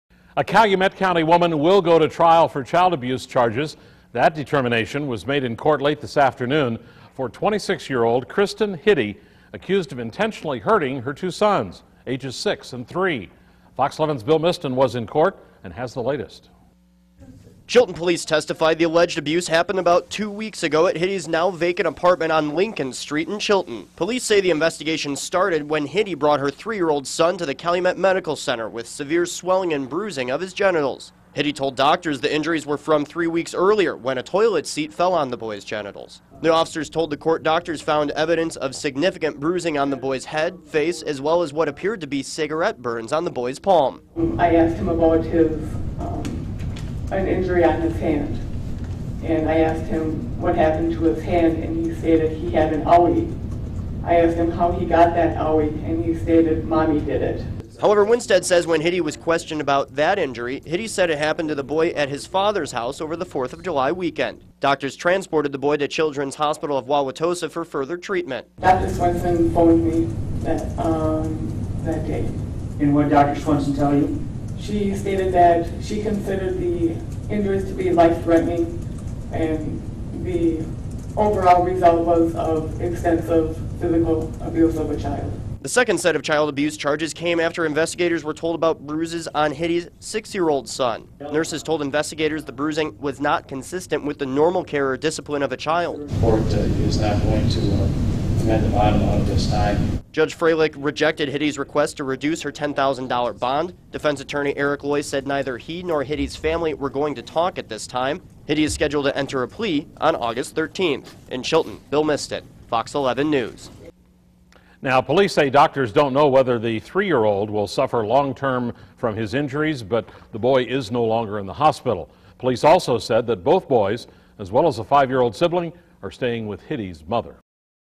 9PM THURS CHILTON CHILD ABUSE
A Calumet County woman will go to trial for child abuse charges.